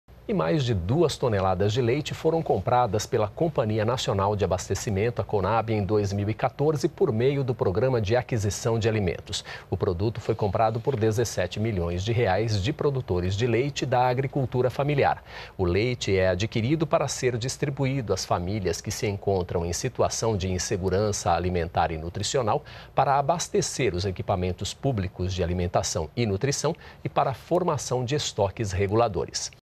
Compradas mais de 2 toneladas de leite da agricultura familiar
NBR NOTÍCIAS - 06.02.15: A Companhia Nacional de Abastecimento (Conab) comprou mais de 2 toneladas de leite em 2014, por meio do Programa de Aquisição de Alimentos (PAA). O produto foi comprado por R$ 17 milhões de produtores de leite da agricultura familiar.